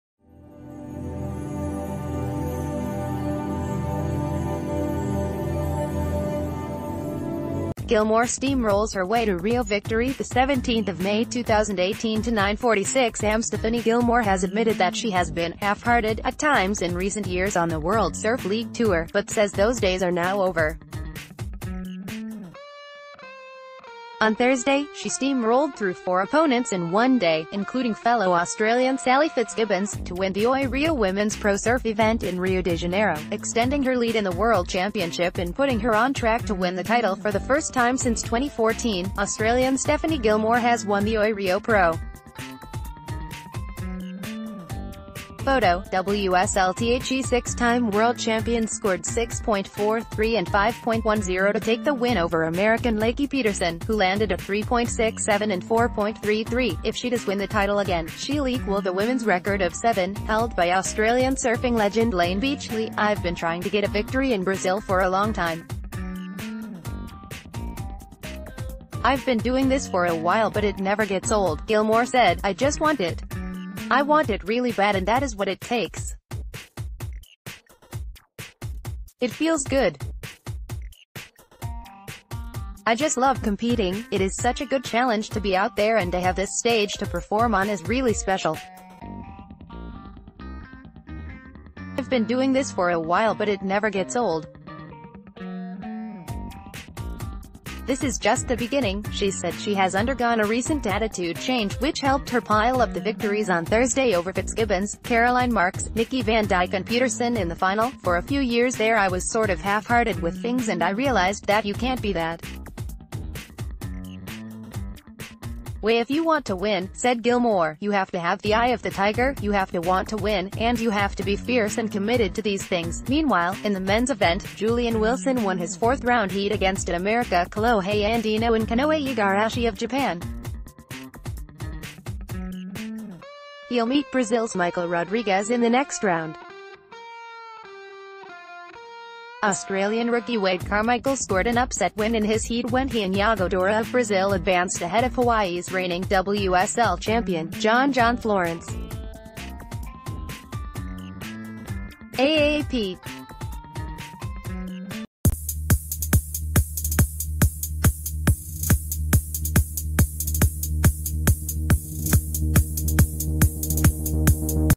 Breaking News | Gilmore steamrolls her way to Rio victory
Gilmore steamrolls her way to Rio victory17 May 2018 — 9:46amStephanie Gilmore has admitted that she has been "half-hearted" at times in recent years on the World Surf League tour, but says those days are now over. On Thursday, she steamrolled through four opponents in one day, including fellow Australian Sally Fitzgibbons, to win the Oi Rio Women's Pro surf event in Rio de Janeiro, extending her lead in the world championship and putting her on track to win the title for the first time since 20...